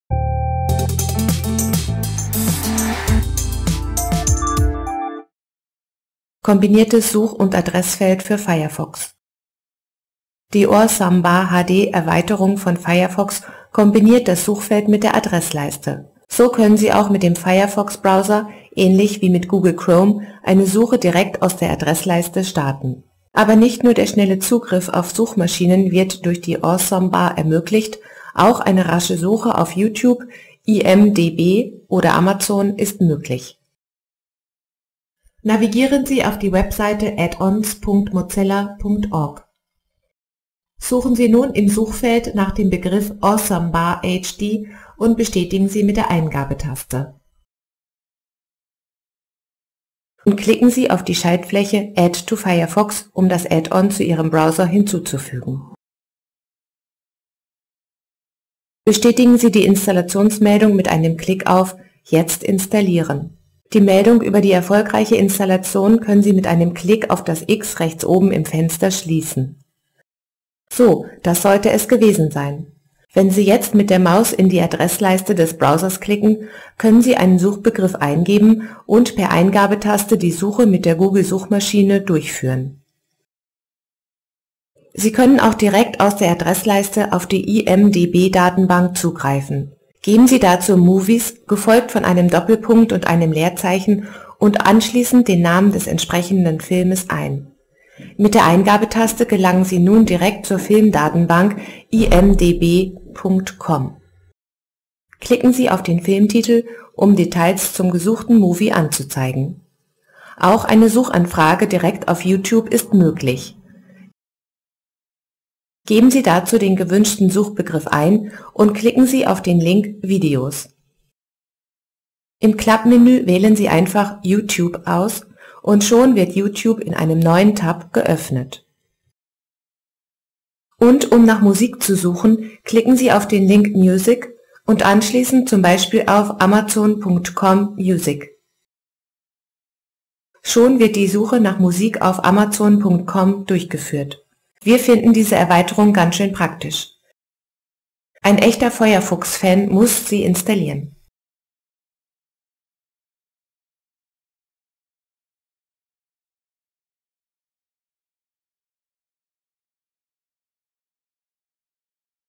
Firefox: Kombiniertes Such- und Adressfeld für Firefox
Die Awesome Bar HD Erweiterung von Firefox kombiniert das Suchfeld  mit der Adressleiste. So können Sie auch mit dem Firefox Browser, ähnlich wie mit Google Chrome, eine Suche direkt aus der Adressleiste starten.Aber nicht nur der schnelle Zugriff auf Suchmaschinen wird durch die Awesome Bar ermöglicht, auch eine rasche Suche auf Youtube, IMDb oder Amazon ist möglich.